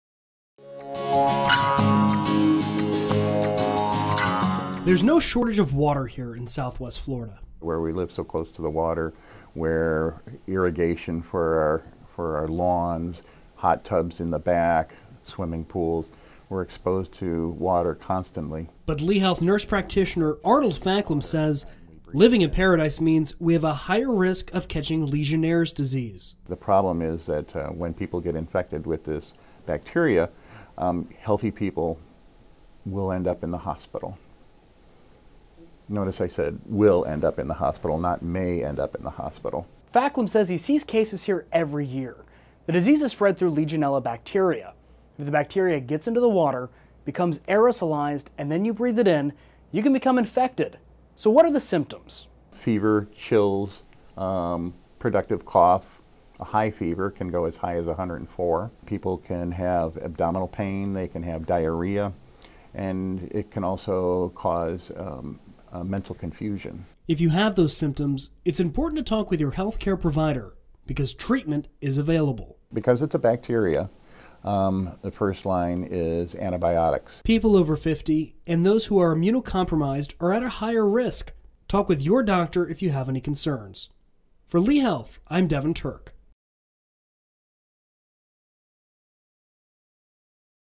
Legionnaires Disease
There’s no shortage of water here in Southwest Florida. “Where we live so close to the water where irrigation for our lawn hot tubs in the back swimming pools. We’re exposed to water constantly,” said Lee Health Nurse Practitioner Arnold Facklam.

Facklam said living in paradise means we have a higher risk of catching Legionnaires Disease. “The problem is when people get infected with this bacteria healthy people will end up in the hospital. Notice I said will end up in the hospital not may.”

Facklam said he sees cases here every year. The disease is spread through Legionella bacteria. If the bacteria gets in the water, becomes aerosolized, and then you breathe it in, you can become infected.

How do you know if you may have it? “You might expect fever, chills, productive cough, fever can go as high as 104 and can also have other symptoms, but besides respiratory people can have abdominal pain they can have diarrhea and it can also cause mental confusion,” said Facklam.

If you have those symptoms, it is important to talk with your healthcare provider because treatment is available. “Because it’s a bacteria, the first line is antibiotics,” said Facklam.

People over 50 and those who are immunocompromised are at a higher risk. Talk with your doctor if you have any concerns.

View More Health Matters video segments at LeeHealth.org/Healthmatters/

Visit LeeHealth.org